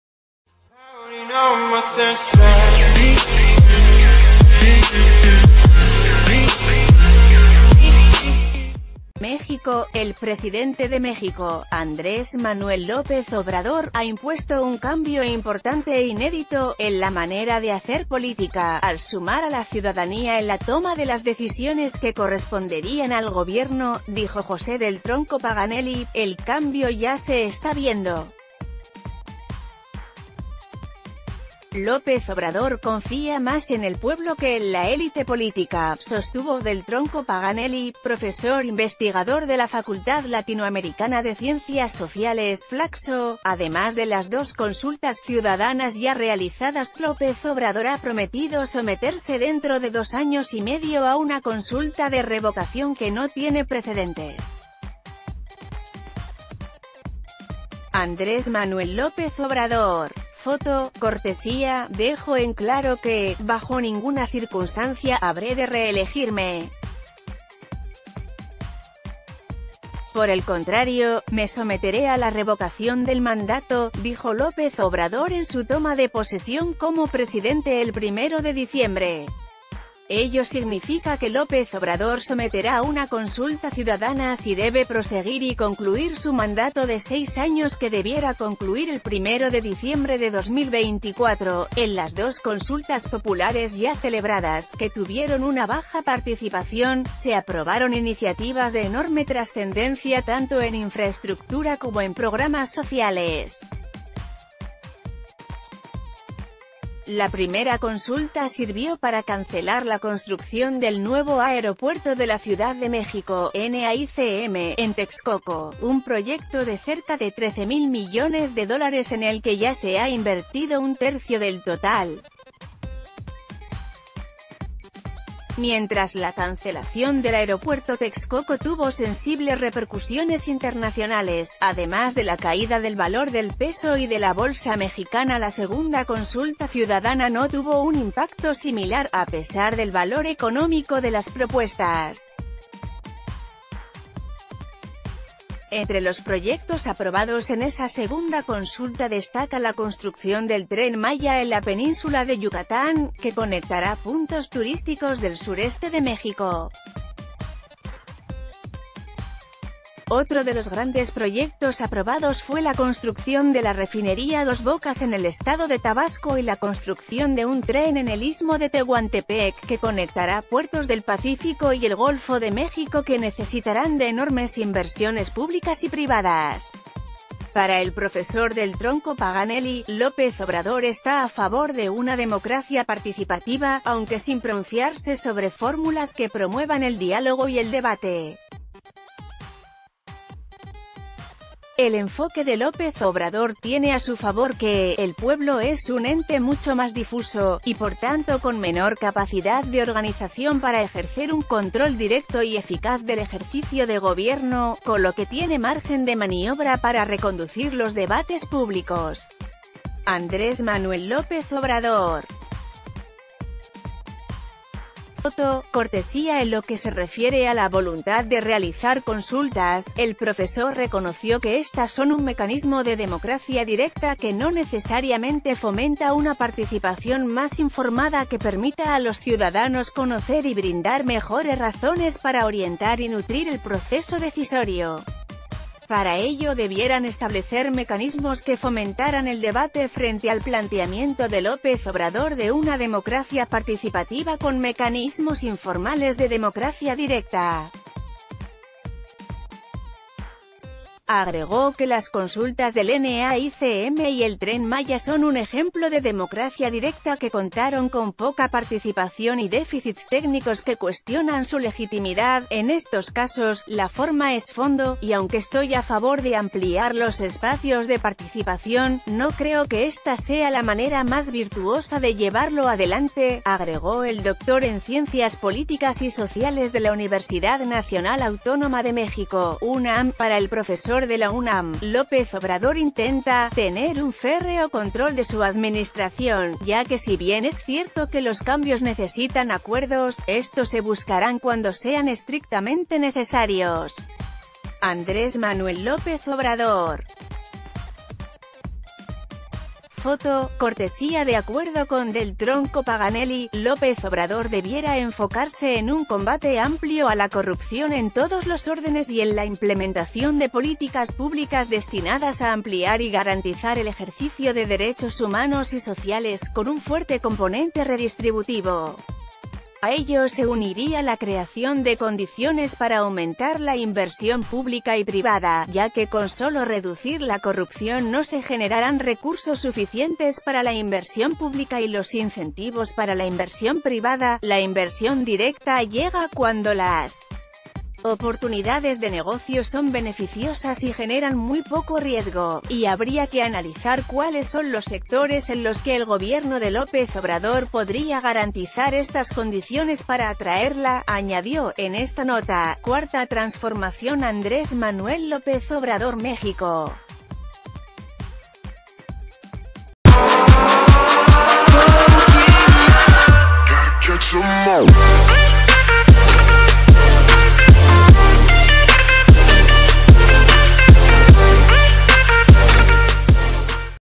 AMLO "confía más en el pueblo que en la élite política", señalan
México.- El presidente de México, Andrés Manuel López Obrador, ha impuesto un cambio importante e inédito "en la manera de hacer política" al sumar a la ciudadanía en la toma de las decisiones que corresponderían al Gobierno, dijo José del Tronco Paganelli."El cambio ya se está viendo. López Obrador...